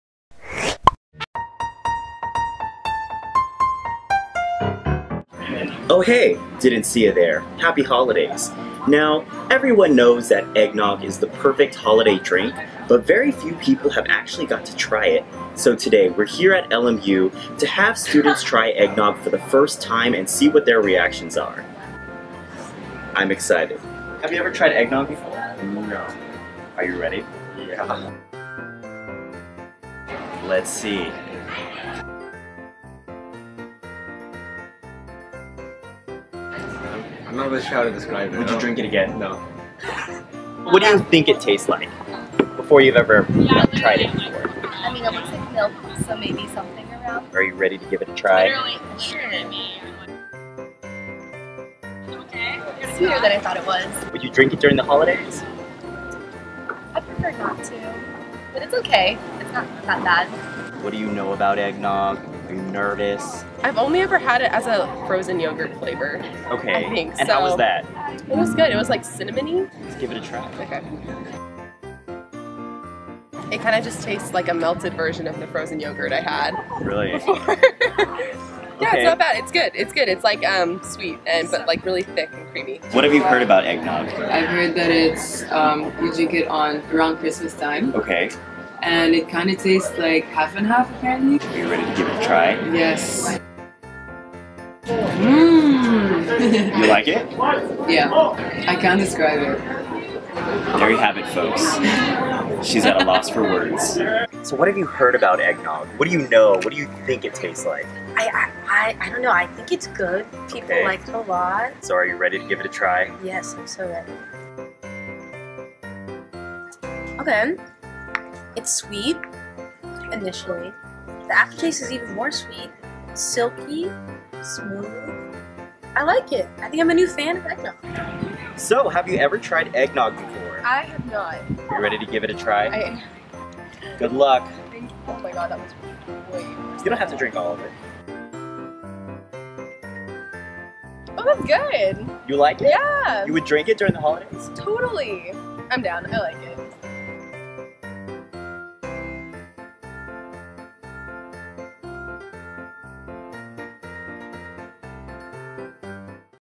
LMU ON: Trying Egg Nog for the First Time
Is Egg Nog the delicious holiday treat we've all heard it is? We let LMU students try it and find out for themselves.